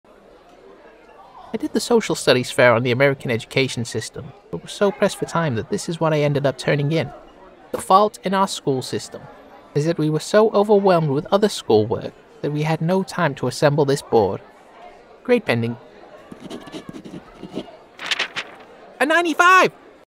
The fault in our school system
A for effort

Hi, Al. This video is a dub of a Tumblr meme about the fault in our school system. Please show it to people who will like it. Thank you.